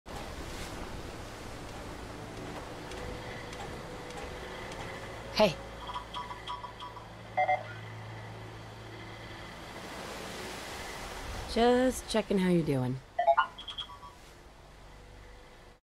Peeper
A short little video of my very cute robot, Peeper.